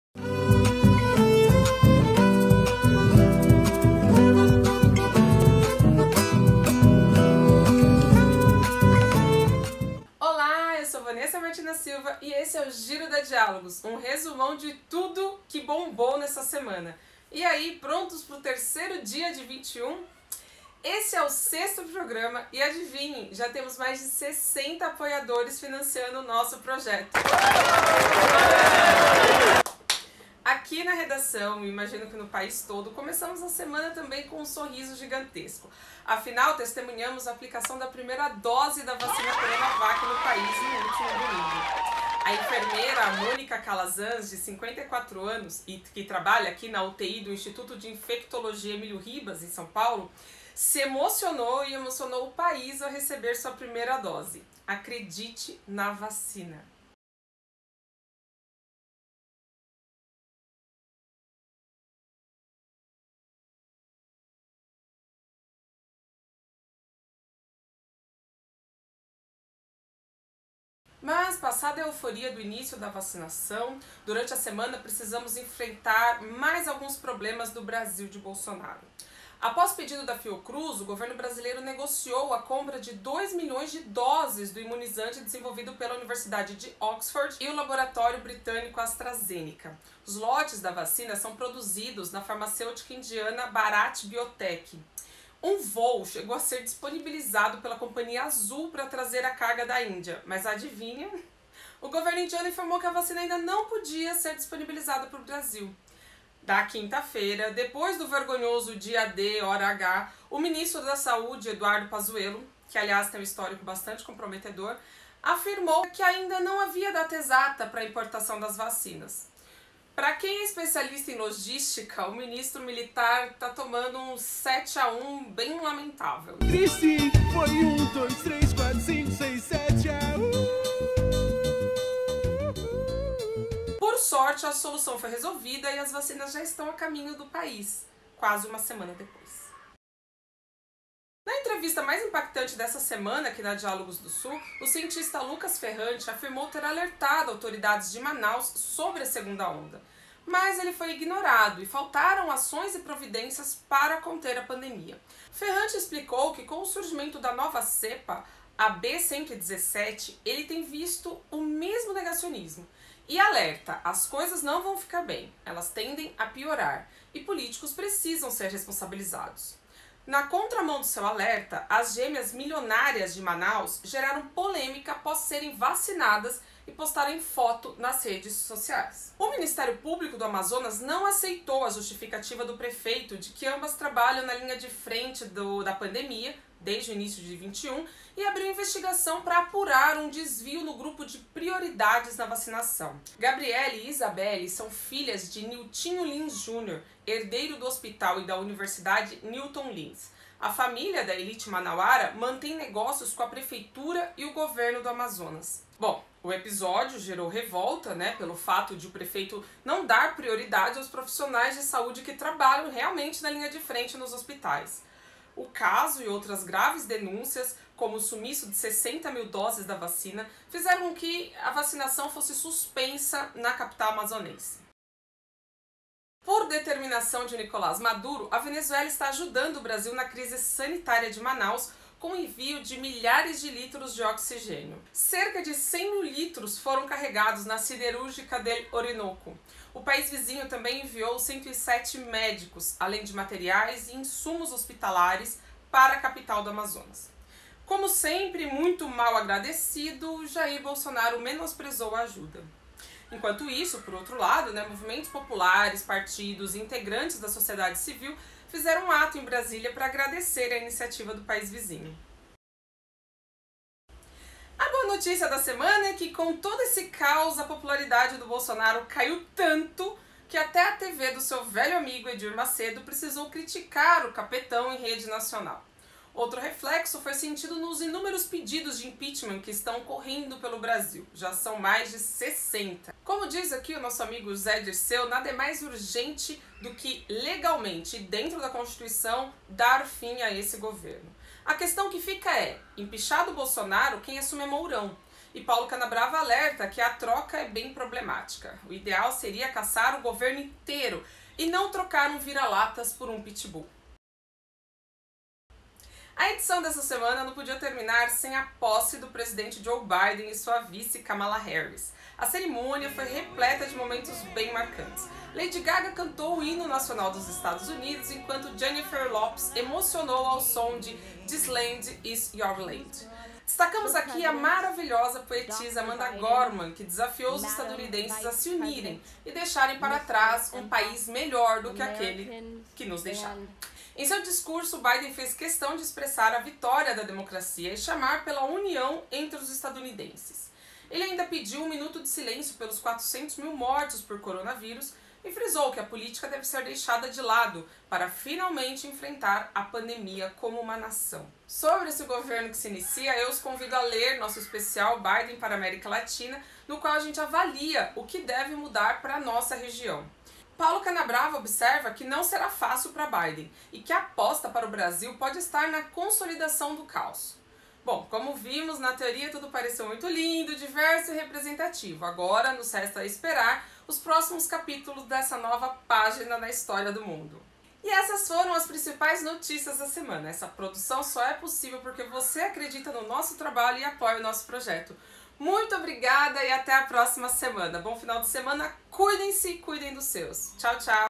Biden é o presidente dos EUA; popularidade de Bolsonaro vai ladeira abaixo
Esse é o Giro da Diálogos, um resumão de tudo que bombou nesta semana.
A boa notícia da semana é que com todo esse caos, a popularidade do Bolsonaro caiu.
A edição dessa semana não podia deixar de falar sobre a posse do presidente Joe Biden e sua vice, Kamala Harris.